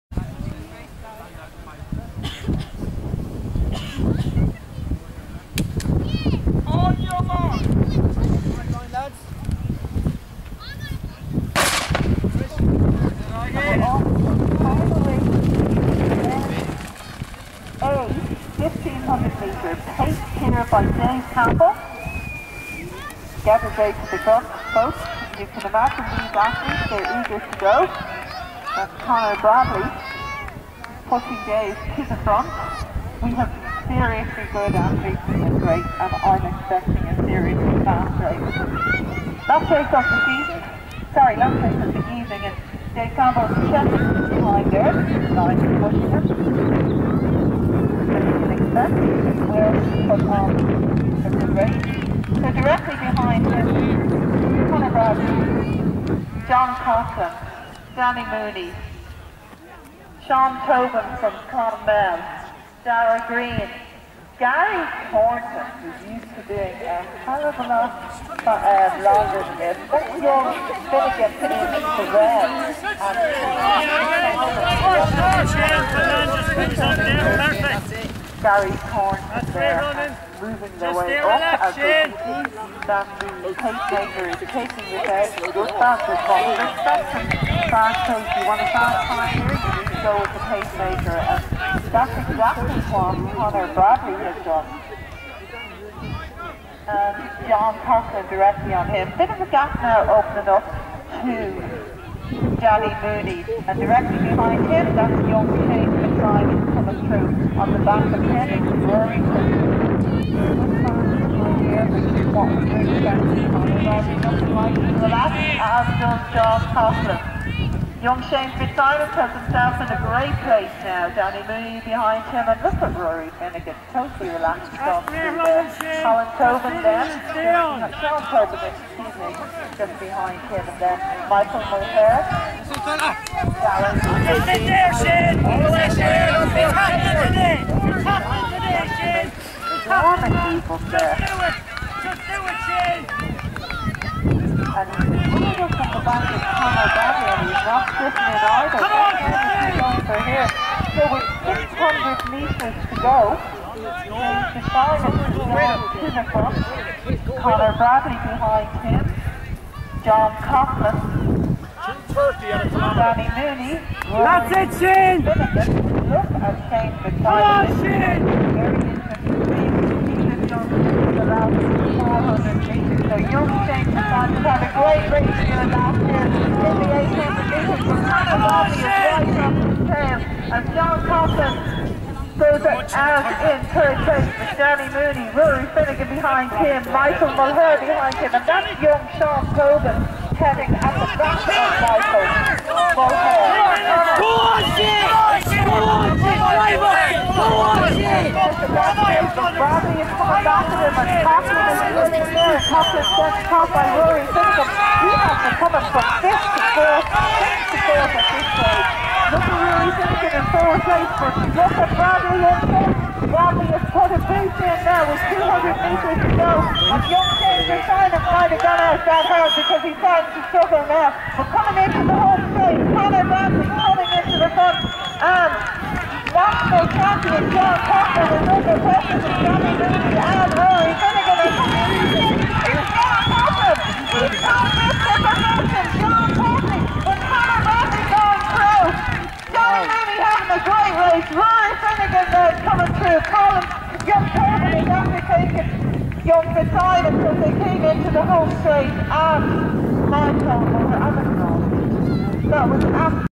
IMC Greystones 1500M A Race 2013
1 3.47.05 John Coghlan MSB AC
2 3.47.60 Conor Bradley City of Derry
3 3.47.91 Danny Mooney Letterkenny AC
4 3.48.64 Ruairi Finnegan Letterkenny AC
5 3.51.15 Sean Tobin Clonmel AC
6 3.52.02 Shane Fitzsimons Mullingar Harriers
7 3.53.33 Michael Mulhare Portlaoise AC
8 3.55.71 Andrew Agnew Annadale Striders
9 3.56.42 Richard Owens UCD AC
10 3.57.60 Gary Thornton Galway City Harriers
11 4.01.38 Edgars Sumskis Clonliffe Harriers